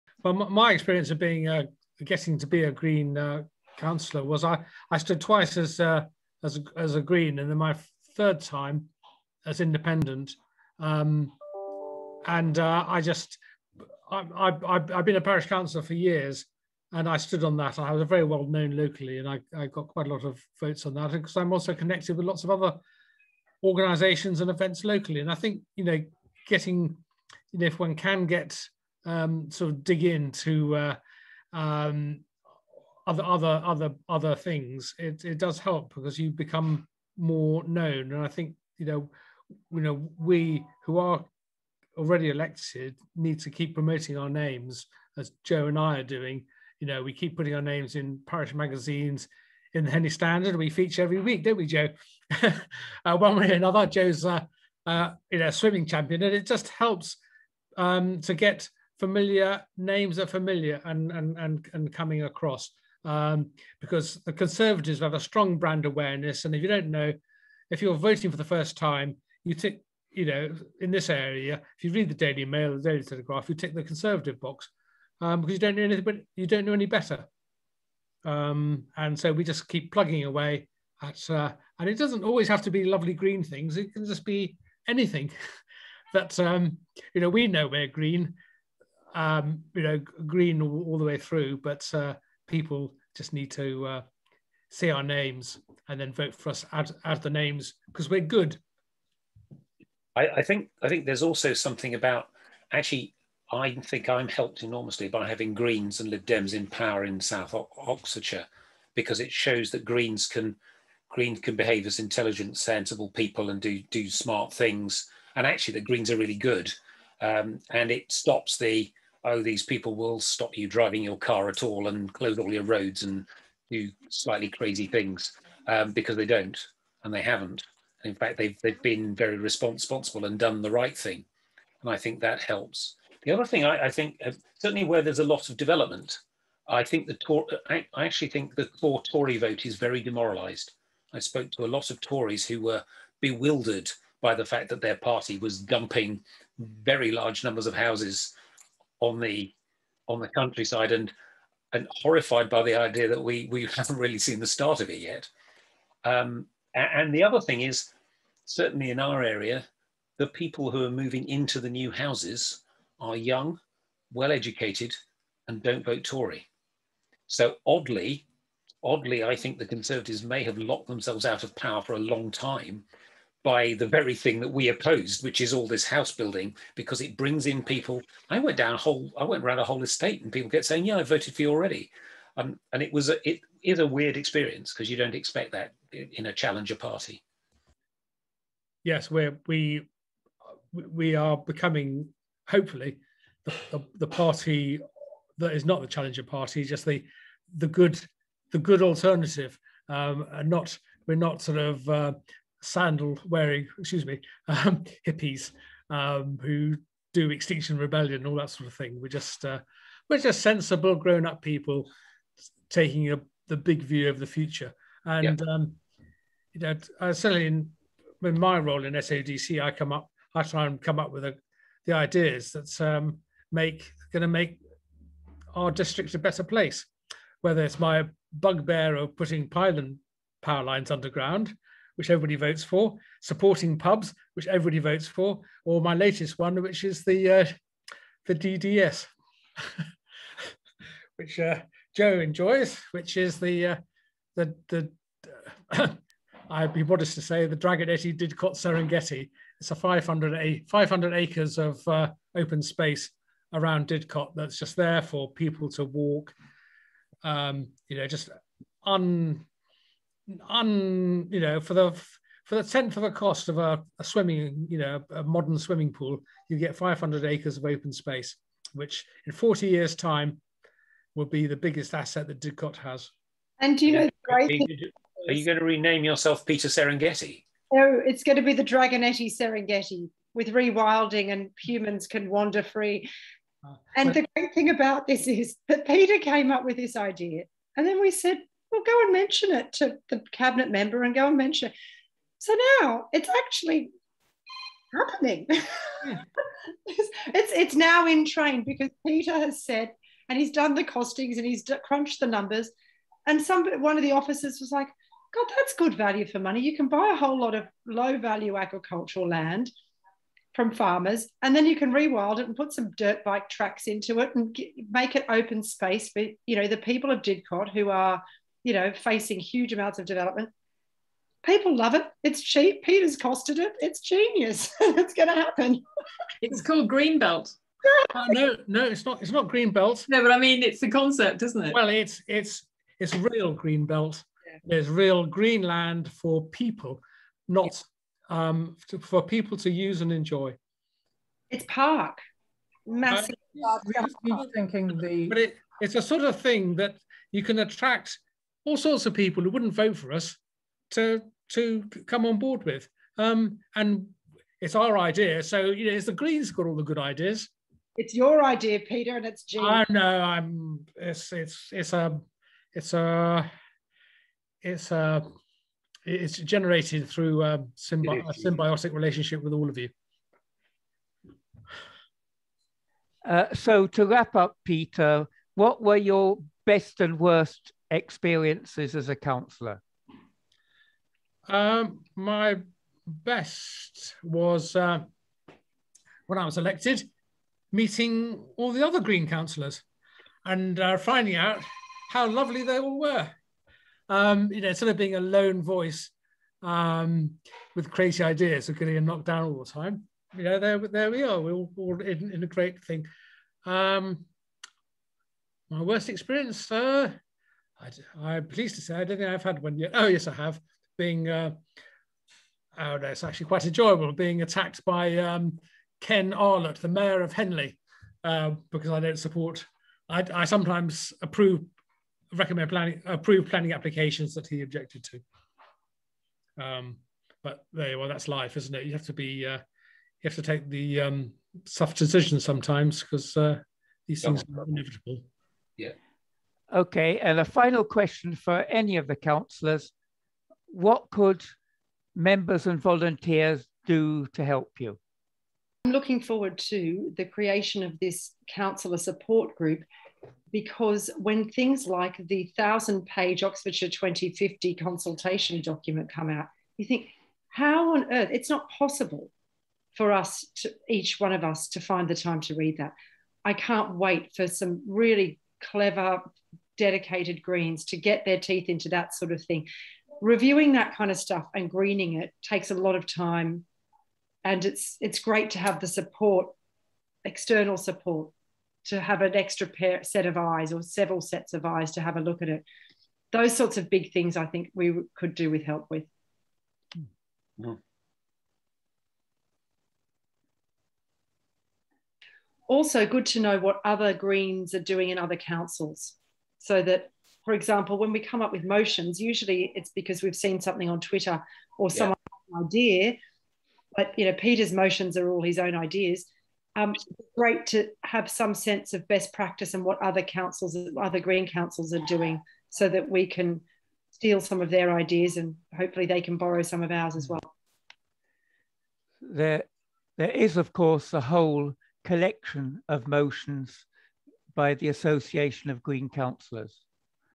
South Oxfordshire Green councillor Peter Dragonetti on his story and rewilding the district
At a meet a Green councillor meeting, Peter Dragonetti talked about finally getting elected as a South Oxfordshire councillor after years of trying, and what he has done as a councillor, including getting the council to adopt a rewilding policy. Other SODC councillors reacted, explaining how the council's politics have been transformed and asking for volunteers to help them cope with active government.